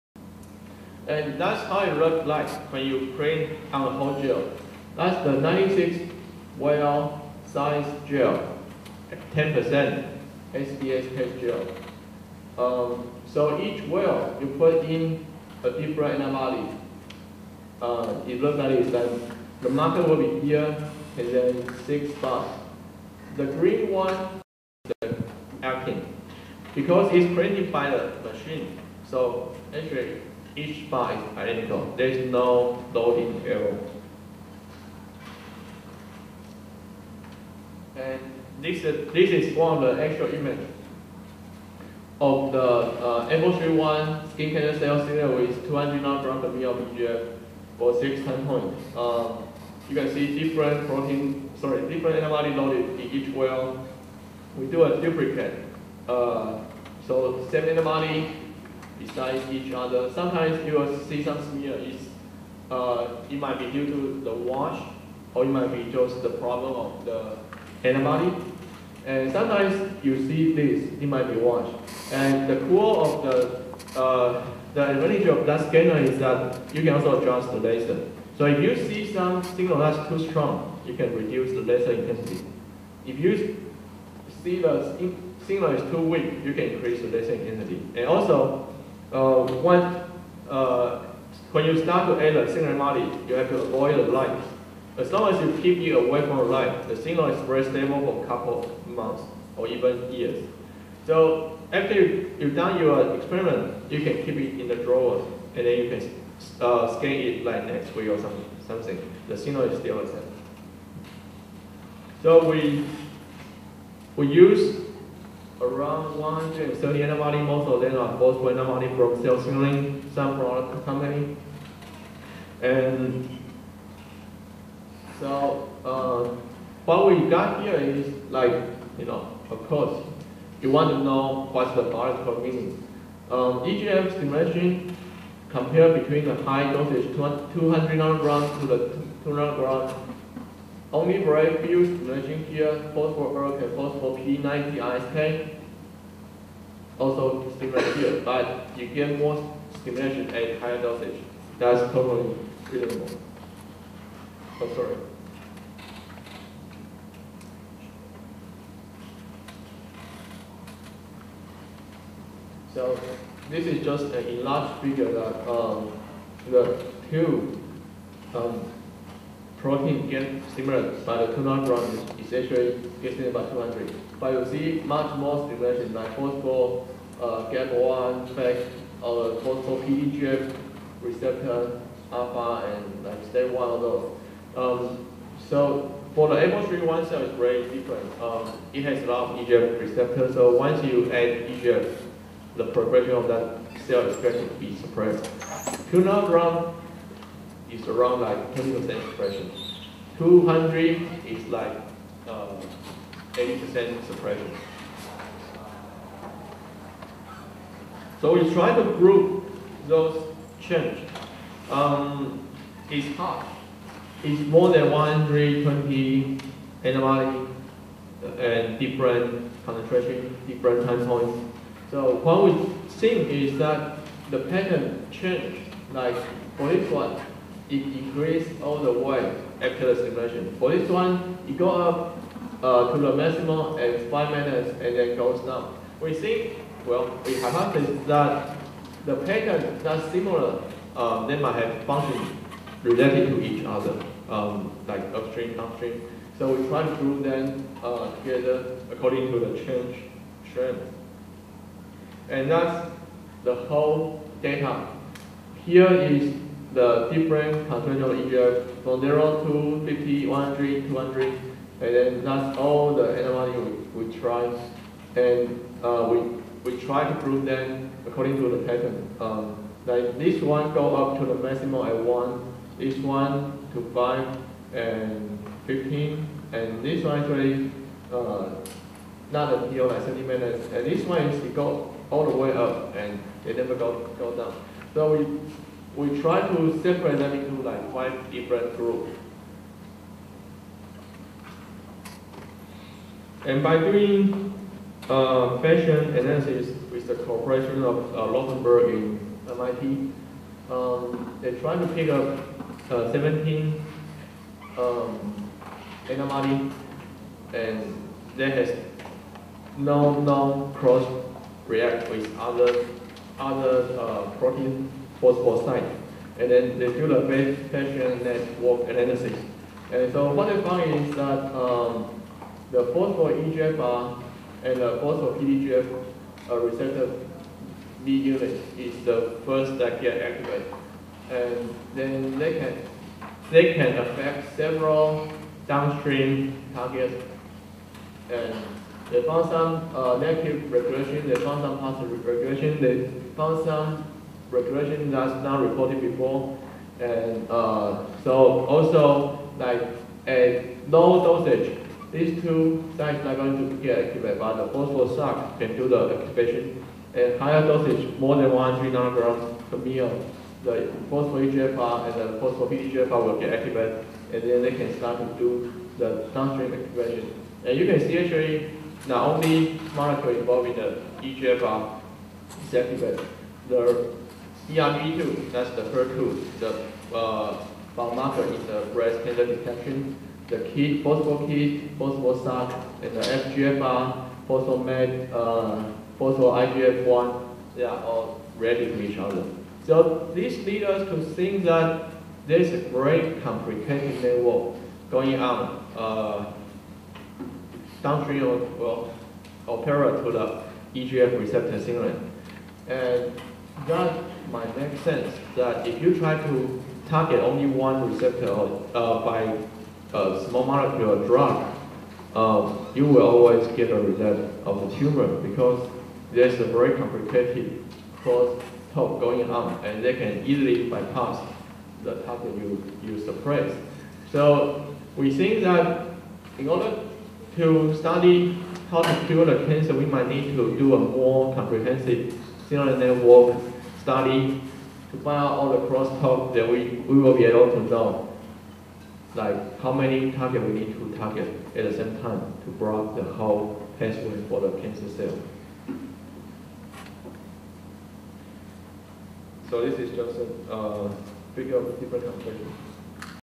Microwestern Arrays - Lecture Series 3
) - Lecture series 3 demonstrates Dr. Chuu’s microwestern array experiments in details and also explains his results using different analyses. Conference: 8th Bioinformatics and Systems Biology in Taiwan (BIT) Systems analysis of signaling pathways in human cancer cells with high-throughput western blotting assay-the microwestern arrays (Dr. Chih-Pin Chuu). Dr. Chuu developed the Microwestern arrays, a high throughput western blotting assay, which enables quantitative, sensitive and high throughput assessment of protein abundance and modifications after electrophoretic separation of microarrayed cell lysates. More videos at Abnova